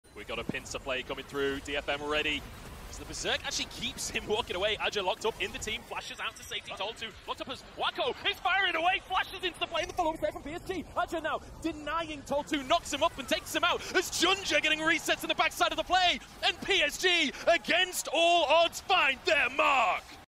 PSG Wako is firing away and flashes in!
PSG vs. DFM | DAY 1 | MSI 2023 | PSG Talon vs. DetonatioN FocusMe | GAME 2 (2023)

MSI 2023, LIVE FROM COPPER BOX ARENA,
IN LONDON, UK.

PLAY-INS STAGE: 5/2-5/7
BRACKET STAGE: 5/9-5/21